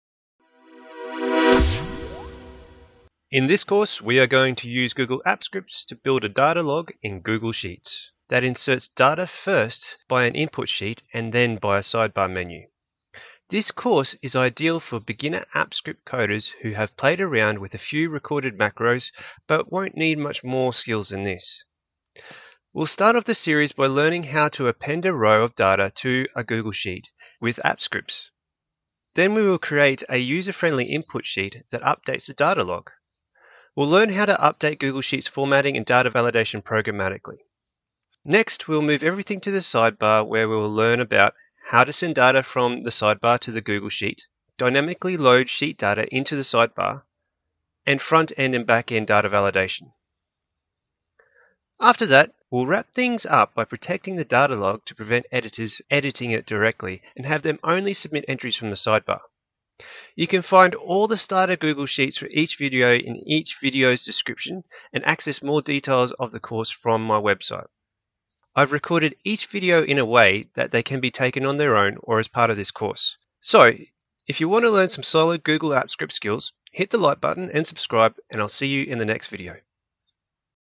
How to build a data log in Google Sheets with an input sheet or sidebar using Google Apps Script
Learn how to create an interactive Google Sheets data log with either an input sheet tab or a sidebar using Google Apps Script.

In this beginner-friendly 9-part course will cover a lot of the basics of how to use Google Apps Script to extend a Google Sheets project including:

- Appending, extracting and inserting data into a Google Sheet.
- Formatting and data validating cells and ranges programmatically.
- Creating custom menu items.
- Building and styling sidebars.
- Dynamically building sidebar from Google Sheet Data.
- Validating input data programmatically.
- Validating data on the client-side to improve user experience.
- Adding data to a sheet tab from a sidebar when the tab is protected.
- Understanding Properties Service and Lock Service.
- A bunch of useful JavaScript methods

This tutorial is part of the: Building a Data Log in Google Sheets with Google Apps Script series.